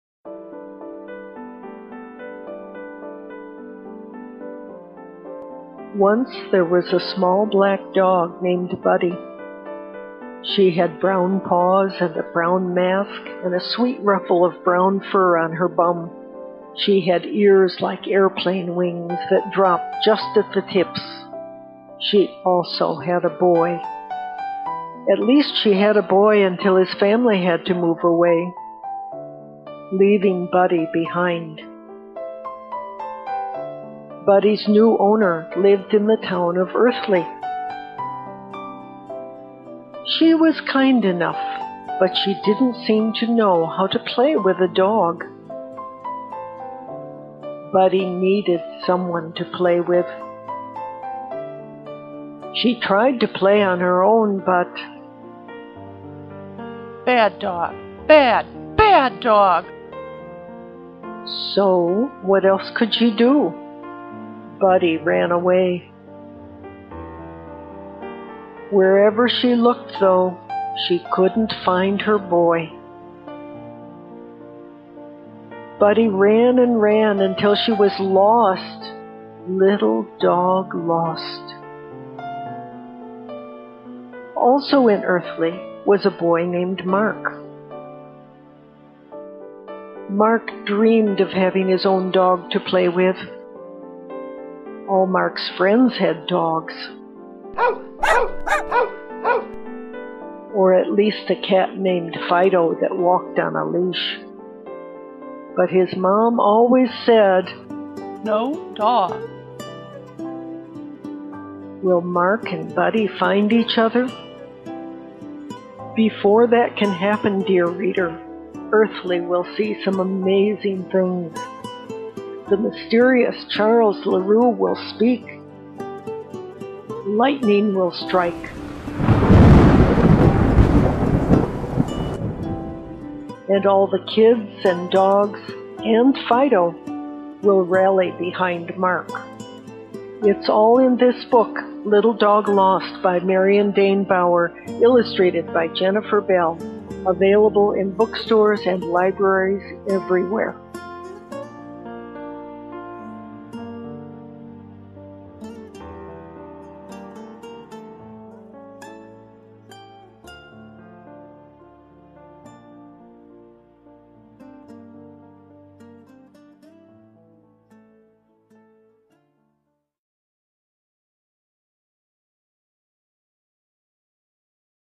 Little Dog, Lost
Written by Marion Dane Bauer. Illustrations by Jennifer A. Bell.

Mark is a boy who needs a dog. But he can't get his mom on board with his plan.

Buddy is a dog who needs a boy. Buddy has an owner already, but not one who understands the kind of love and care—the "something more"—a dog needs.

Mr. LaRue is a neighbor who needs a community. He's alone all the time in his huge old house—and everyone needs more than that.

Over the course of a summer thunderstorm and one chaotic town council meeting, these three characters cross paths and come together in a timeless tale ripe with emotions and told in verse that resolves with love, understanding, and a sense of belonging.

Video services provided by Winding Oak.